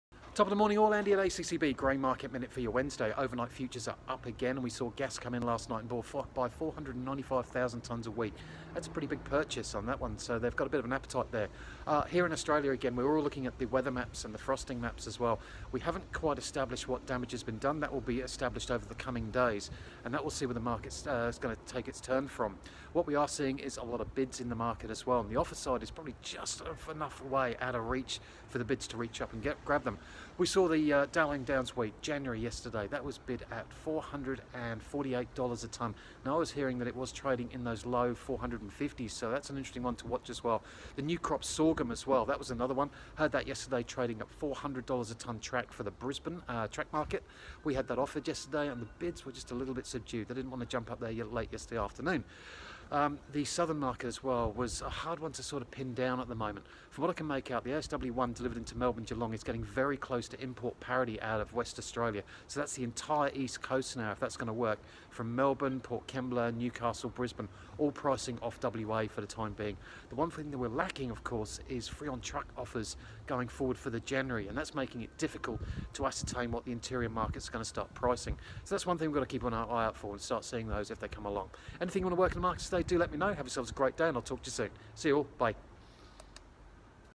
Grain market Australia
ACCB grain market minute Dec Wheat futures +$1 Aussie market printing higher values across the board yesterday Aud firmer this morning takes away some of the futures gains all eyes on rainfall and temperature maps